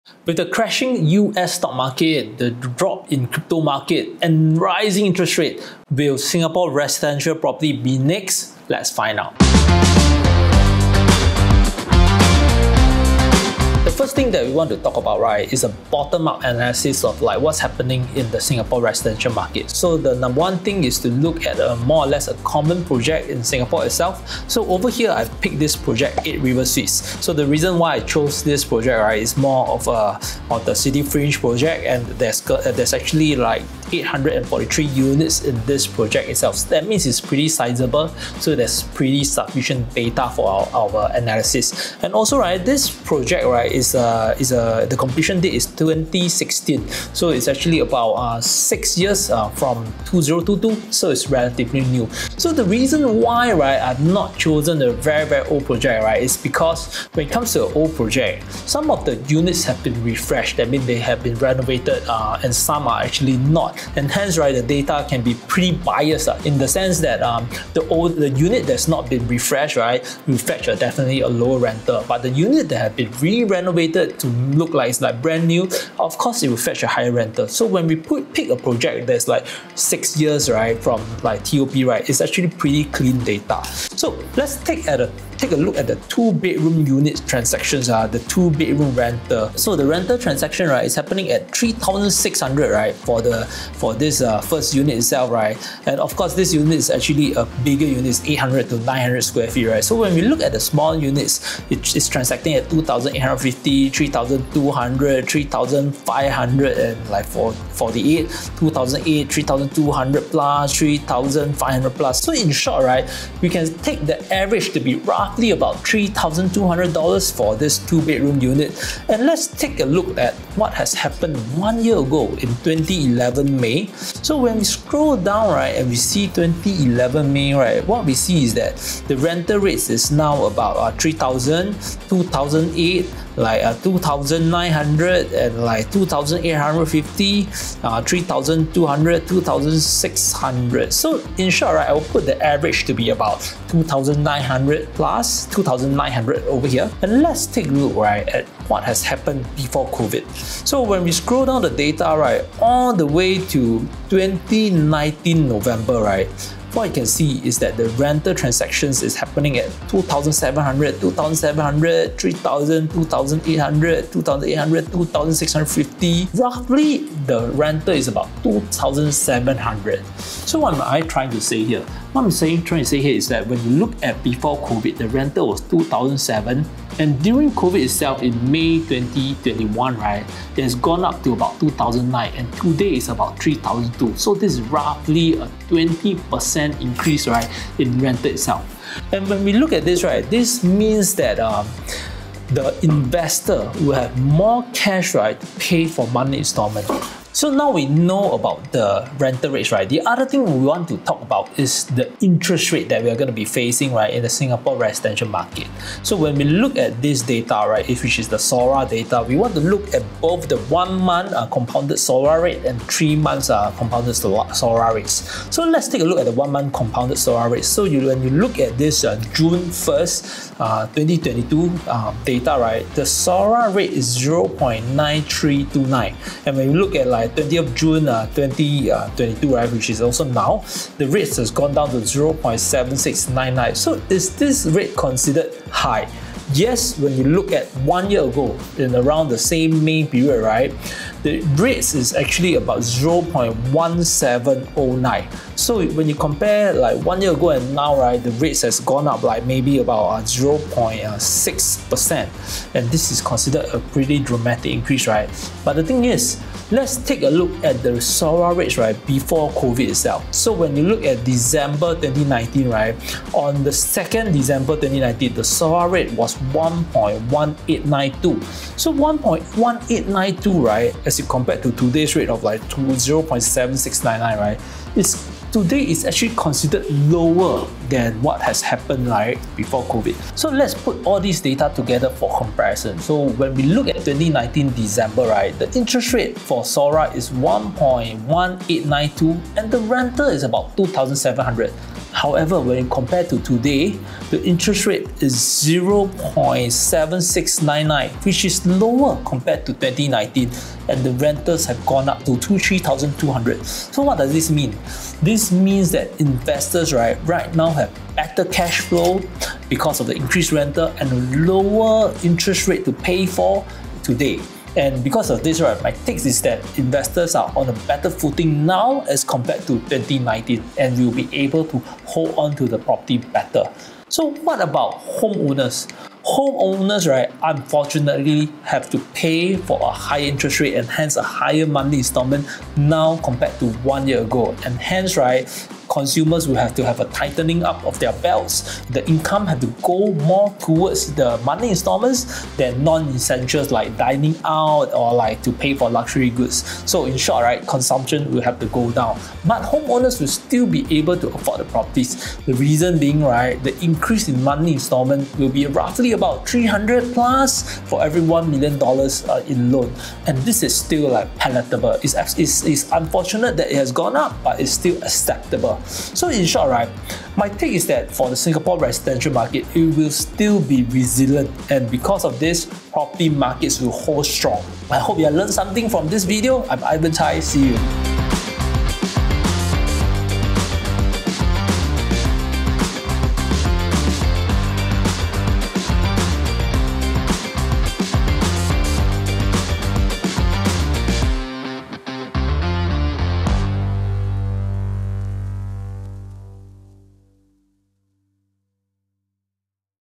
Can 🇸🇬 Residential Market Survive The Crash? | I Quadrant
Amidst high inflationary environment, investing in property has been a controversial topic. While it's true that we have to fork out more capital due to higher interest rate, property prices will get inflated over time, which means capital appreciation for us in the long run.

Aside from interest rate and capital appreciation, there are more on-the-ground indicators we should take into consideration. In this episode, Ivan will bring us through the importance of Rental Rate and Loan Interest Rate in understanding 🇸🇬 property market better. See you.
Let us know your thoughts and queries in the comment section below. Together we learn and achieve something greater along the journey :)